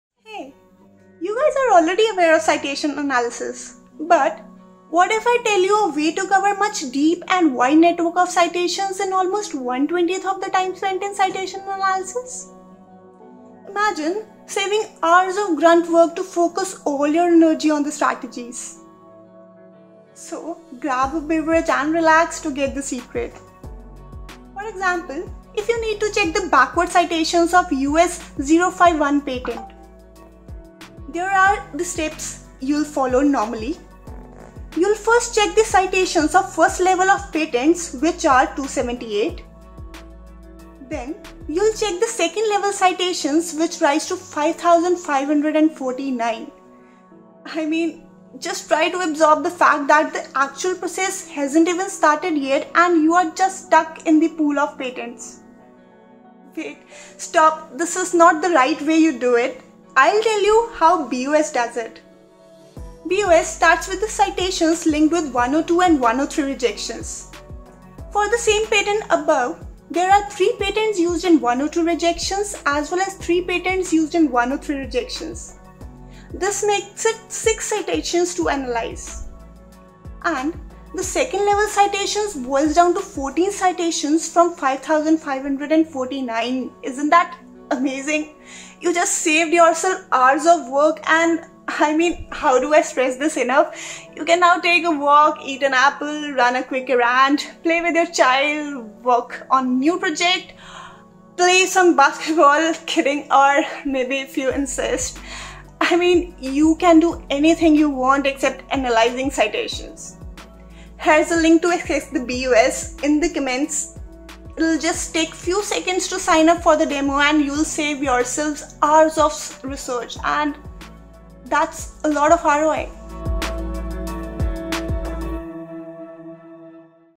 Stuck in the pool of patents doing Citation Analysis?
Imagine saving hours of grunt work and focusing all that energy on making strategies. Fascinating?

Well then here’s a productivity enhancer for you that will aid you to cover a wide and deep network of citations in 1/20th of the time you used to spend!

In this video, Goury Sikka introduces a time-saving tool for citation analysis and explains the difference between the usual citation analysis and the one using the tool!